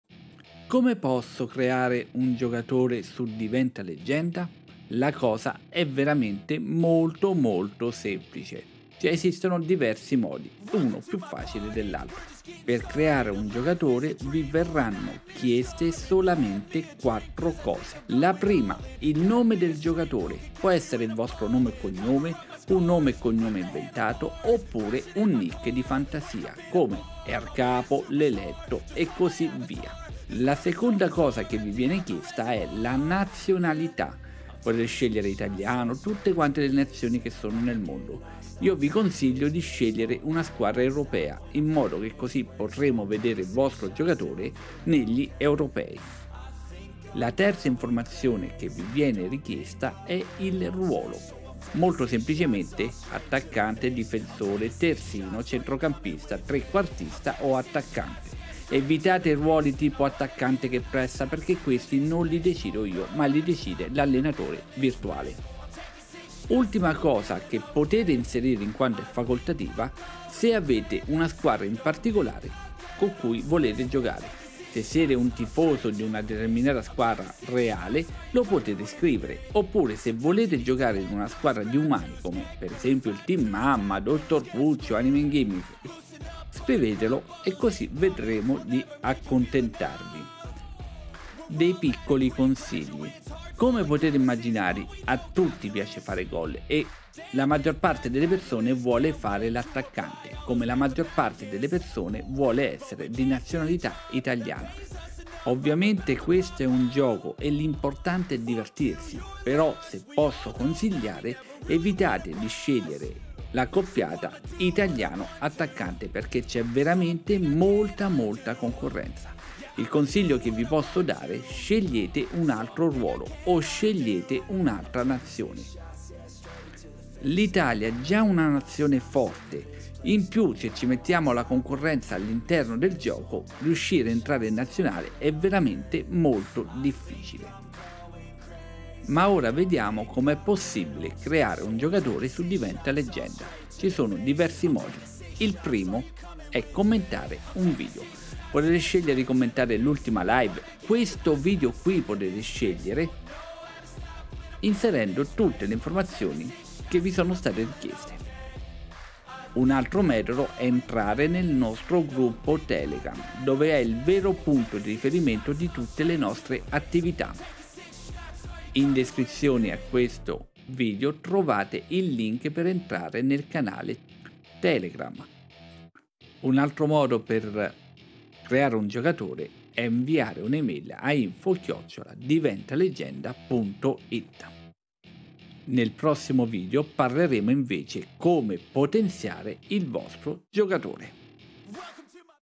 Come creare un giocatore - Gioco di ruolo calcistico
Un breve video che spiega cosa è Diventa Leggenda

OPPURE COMMENTA IL VIDEO

Crea il tuo giocatore gratuito